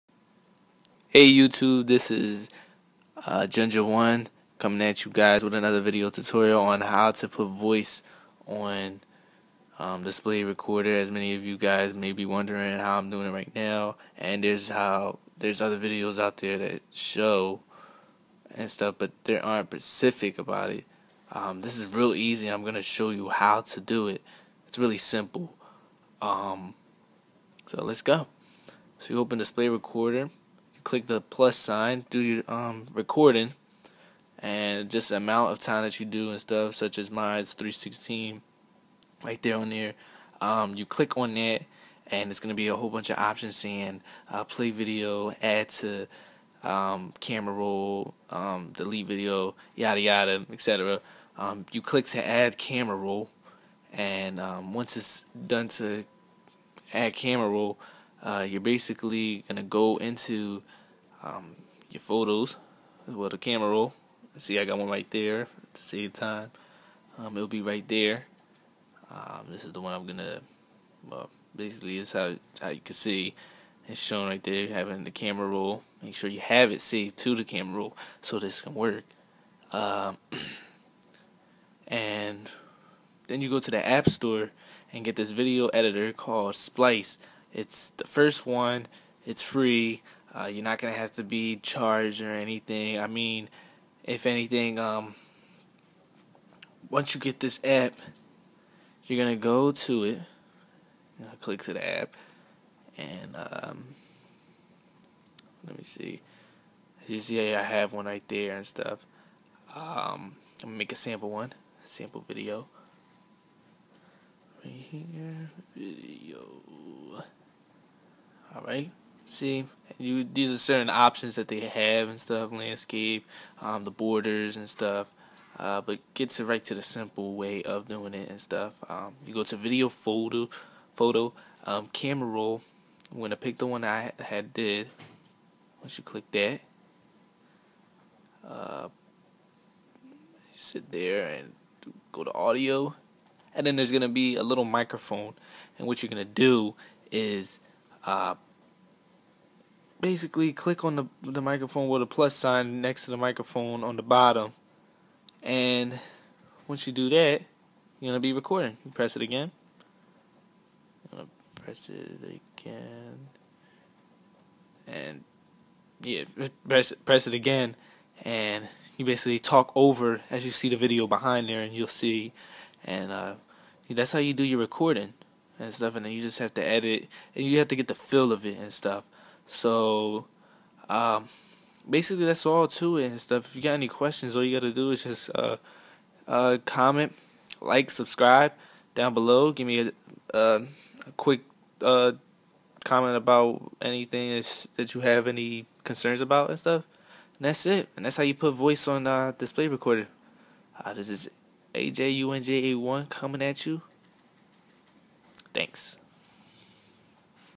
How To Get Voice on Display Recorder
Cydia Update - Display Recorder now supports voice!
This a video tutorial on how to put your voice on Display Recorder.
You can also put music on it too.
This is a very easy process and you do need a JAILBROKEN iPhone.